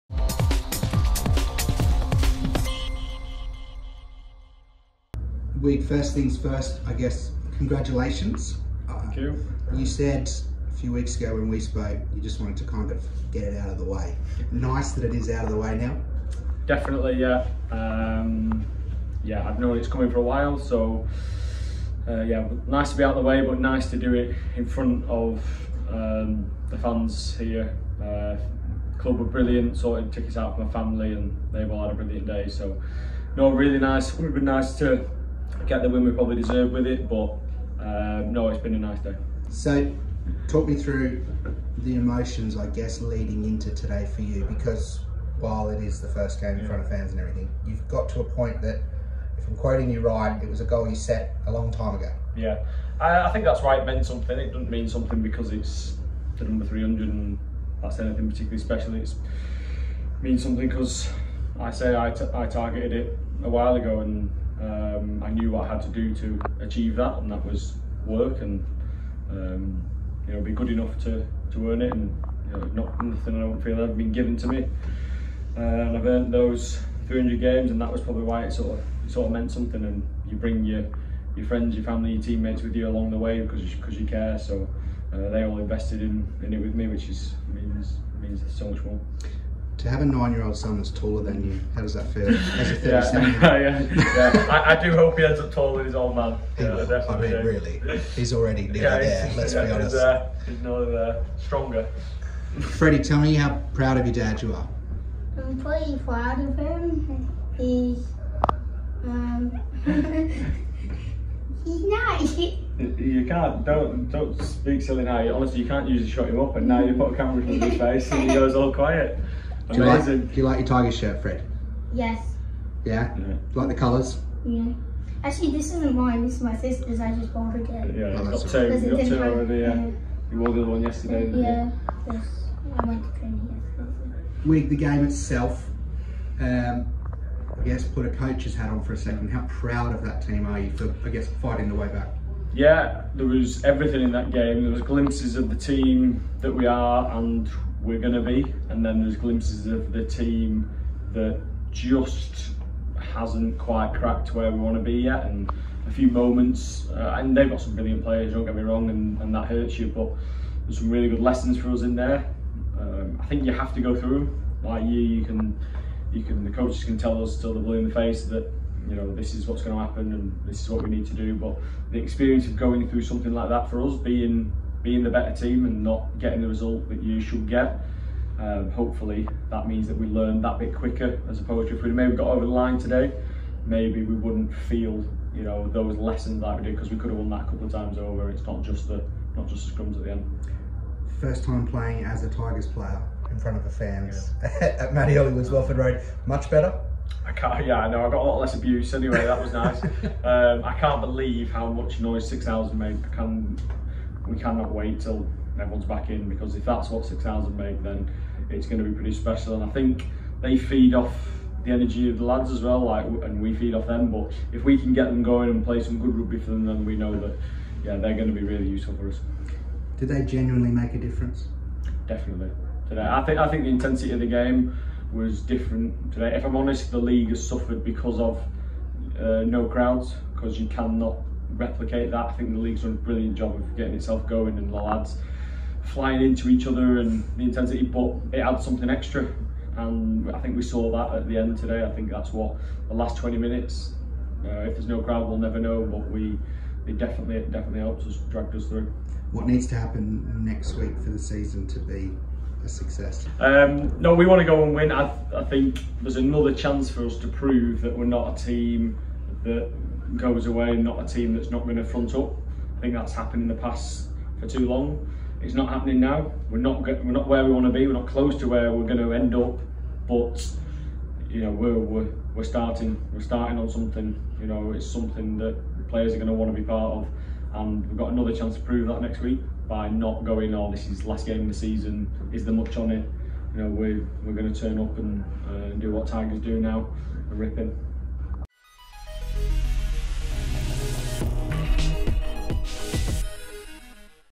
Post-Match Chat: Richard Wigglesworth
After becoming the first player in the competition's history to make 300 Premiership Rugby appearances, Richard Wigglesworth speaks to LTTV.

Wigglesworth came off the bench to reach the history-making milestone in the Leicester Tigers game against Bristol Bears at Mattioli Woods Welford Road in Round 21 of the 2020/21 Gallagher Premiership season.